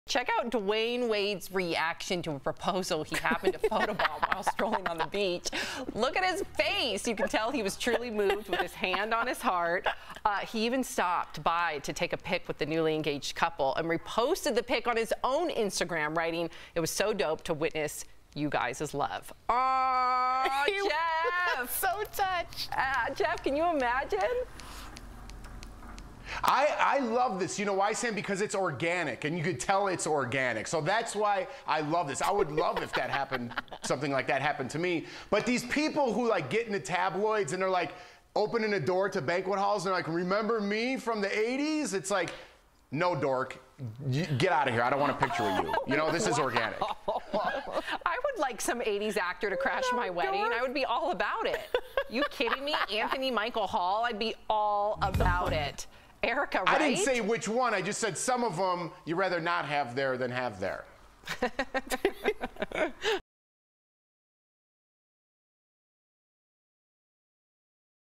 Dwyane Wade Adorably Crashes Marriage Proposal
Miami Heat legend Dwyane Wade found himself walking into a marriage proposal this week on a California beach.

Ryan Basch shared the pictures on his Instagram, showing a surprised Wade with his mouth wide open and his hand over his heart.

How cool is that? Share your DBLtake.